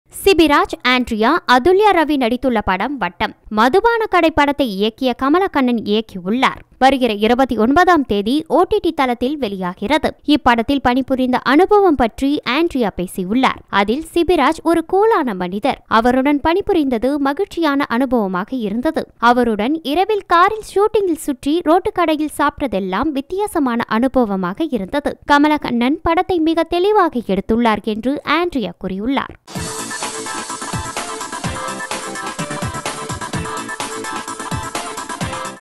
பிரபல நடிகருடன் இரவில் காரில் ஊர் சுற்றும் ஆண்ட்ரியா..! | Andrea Jeremiah
Title : பிரபல நடிகருடன் இரவில் காரில் ஊர் சுற்றும் ஆண்ட்ரியா..!

Content Writer : Gowtham
Voice : GAYATHRI ELUMALAI